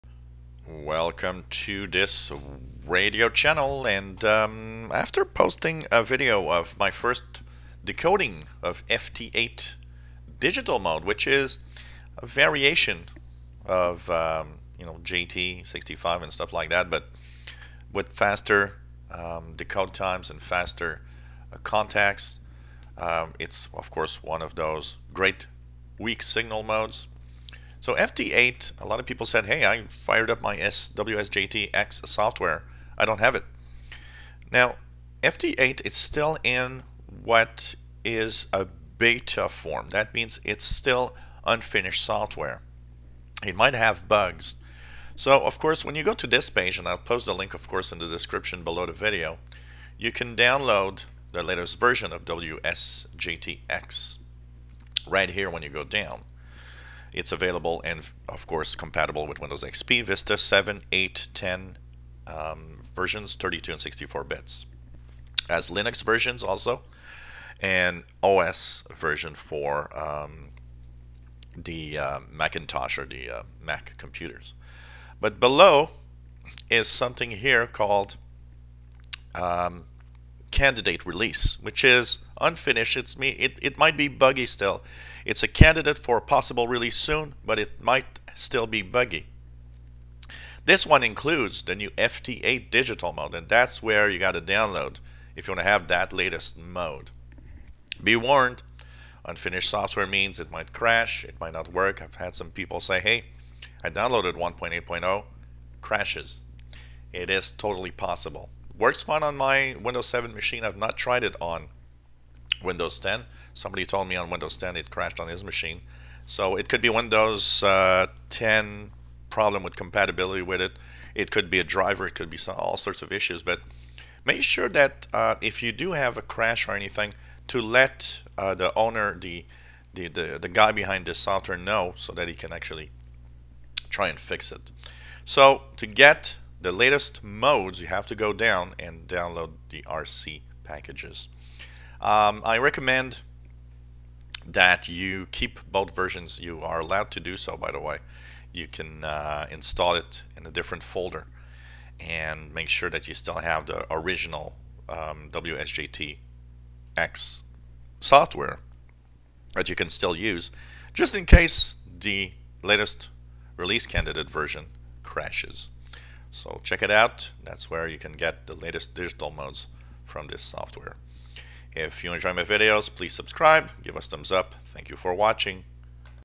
Where to get the latest FT8 Digital mode with WSJT X Software
Move down the page to Release Candidate version to have new features, but be warned, it might crash !